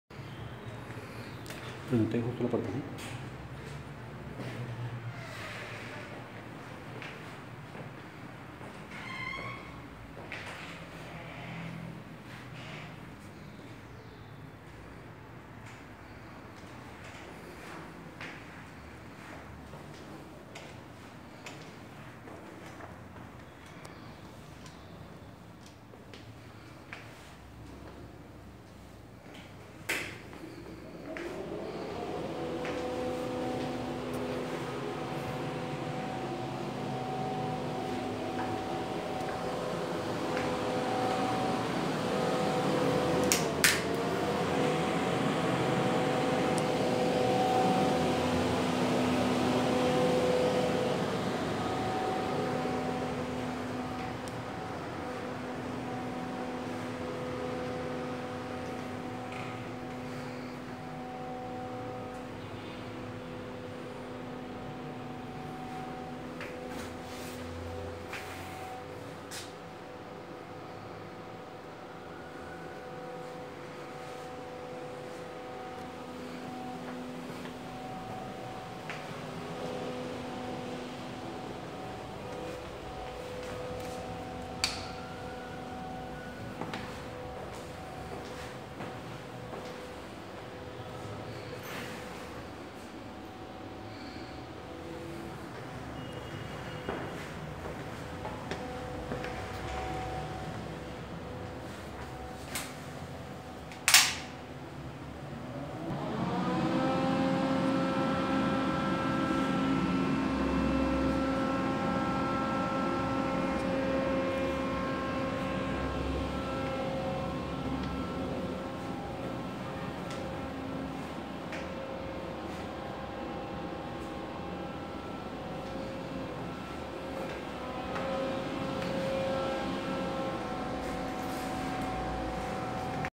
Apartamento en Gran Granada - 2232
Apartamento en arriendo Gran Granada, cuenta con 65 mts. Sala comedor en un solo ambiente, tres habitaciones con sus closet, dos baños con calentador eléctrico, cocina integral mixta, sus pisos son en cerámica, remodelado, ubicado en un primer piso sin ascensor, cuenta con un parqueadero cubierto sencillo. Excelente ubicación con acceso por Cll. 170, Autopista Norte y Av. Suba, cerca a Centros Comerciales y al Portal Norte.